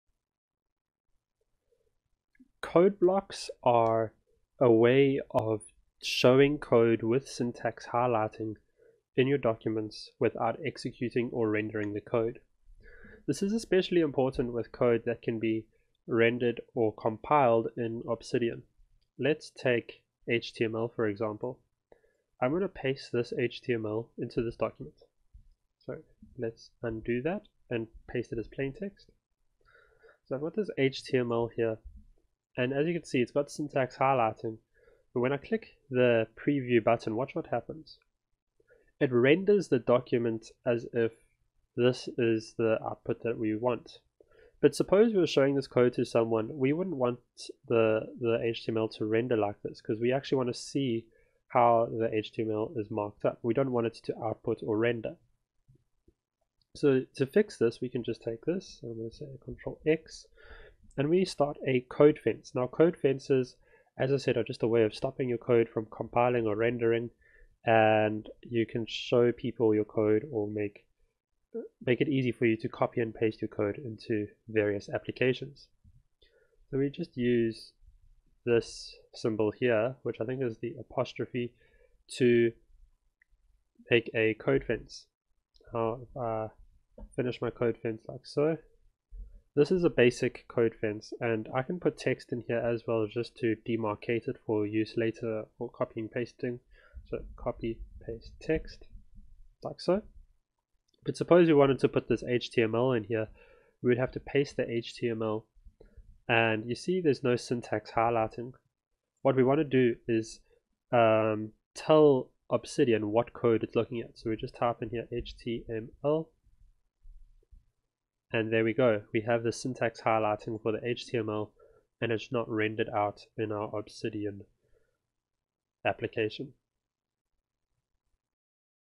Obsidian.md Basics | How to use Code Blocks/Fences
Also known as code fences!

This video is part a beginners obsidian guide, you can view and watch the whole guide here: